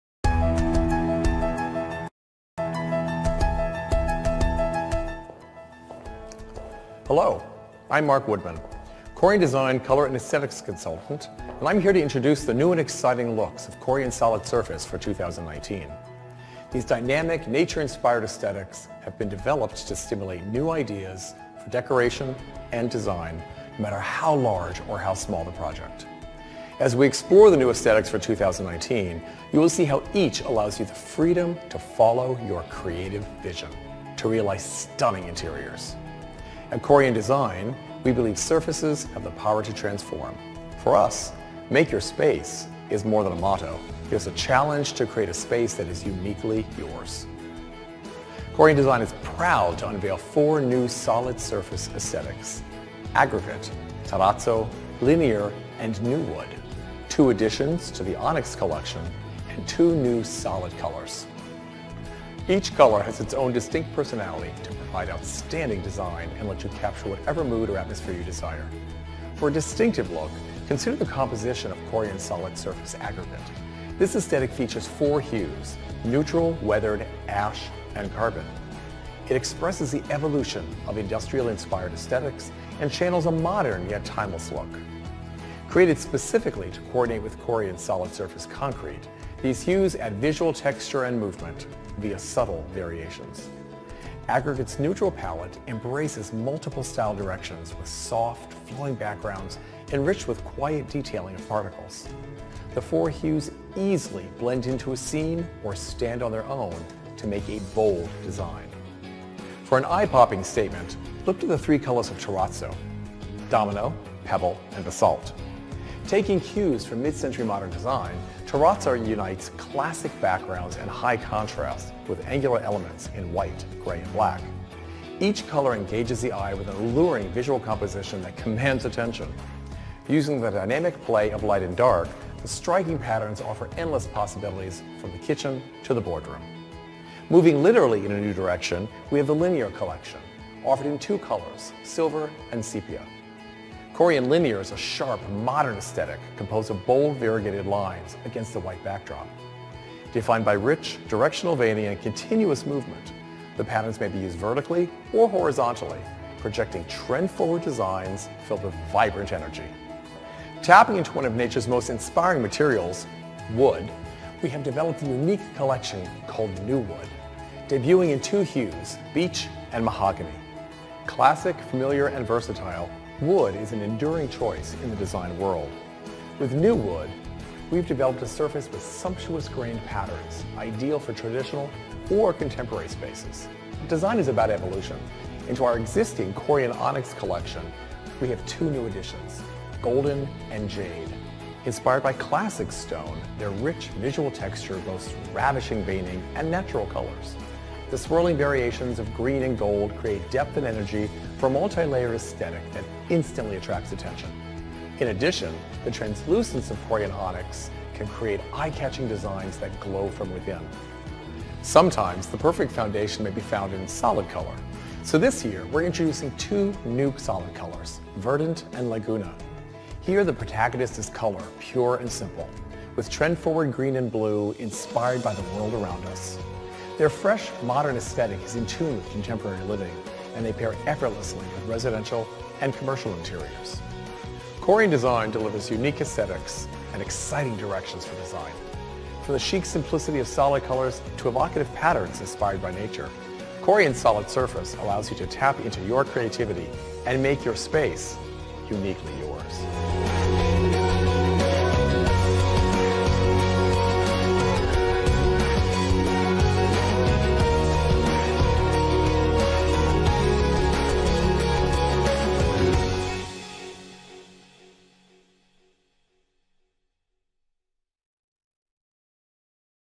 Corian Solid Surface: Màu Đá Solid Surface Mới Năm 2019
Corian solid surface giới thiệu những màu sắc nổi bật của năm 2019, với những thiết kế mang phong cách đổi mới, đầy sáng tạo. Là sự lựa chọn hoàn hảo cho mọi không gian nội thất

Công Ty TNHH Thiết Bị Bảo Kim